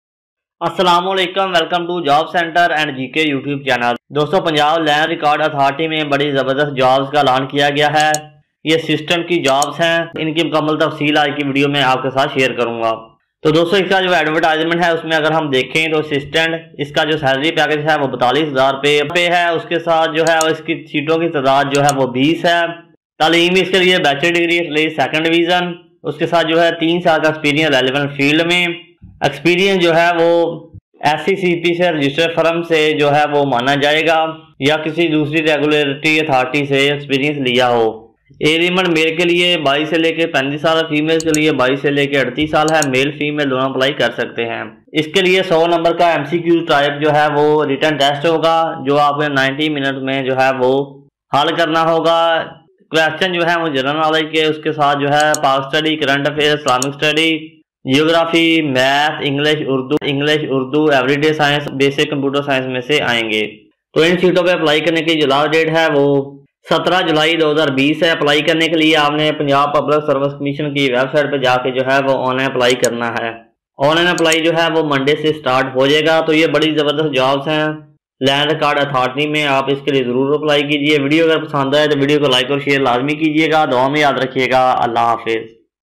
Punjab Land Record Authority Jobs 2020 | PPSC New Jobs Today In Land Record Authority Apply Online
Punjab Public Service Commission PPSC announced jobs for Posts of Assistant in latest advertisement for Punjab Land Records Authority. Punjab Land Records Authority is core department and manage to control and regulate the lands record in Punjab